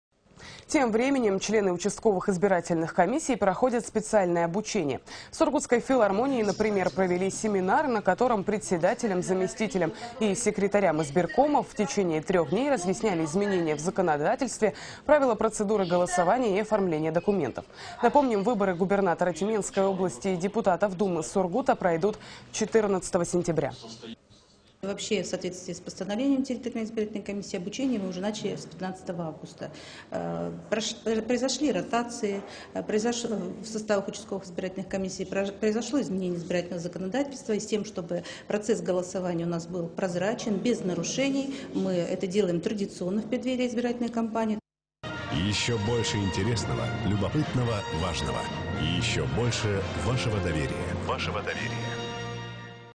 Обучение для выборов (ВЦС от 26.08.14)
Члены участковых избирательных комиссий проходят специальное обучение.